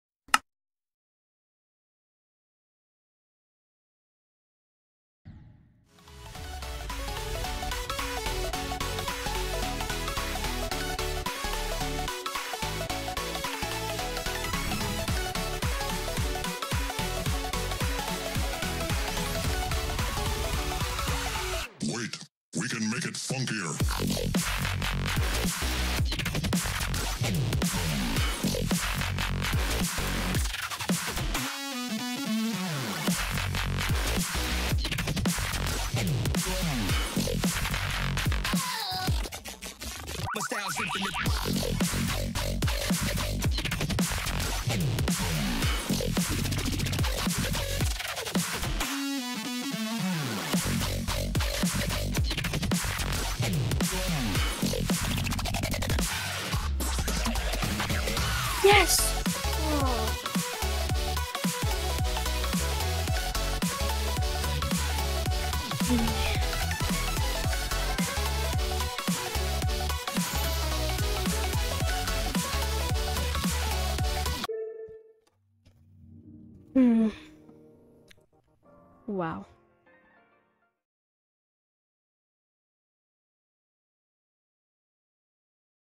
I beat Funked Up with a controller 👍
I just beat funked up with a controller cause my mom says my mouse is too loud 🤷‍♂️